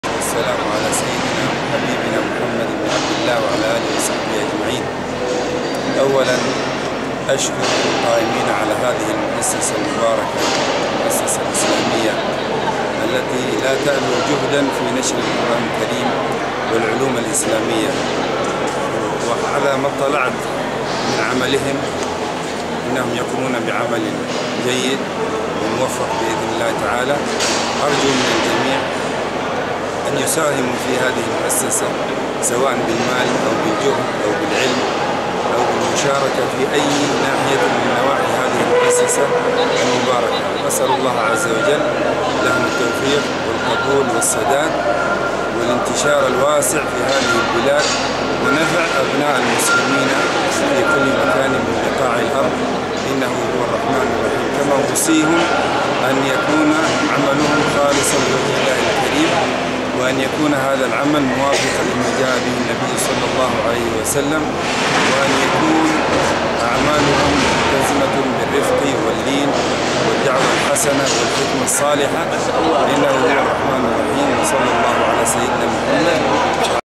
Ch. Ahmed BATIEH soutien la FSIP Faculté des sciences islamiques de Paris
Cheikh Dr. Ahmed BATIEH Représentant de la comission mondiale du Saint Coran / KItab wa assounna (Djeddah) appelle à soutenir  la FSIP Faculté des sciences islamiques de Paris

La Faculté des Sciences Islamiques de Paris (FSIP) est une institution d’enseignement et de formation, spécialisée dans l'enseignement de la langue arabe et des sciences islamiques aux adultes.

Faculté d’Islamologie de Paris - FSIP

11 Rue Ferdinand GAMBON, 75020 Paris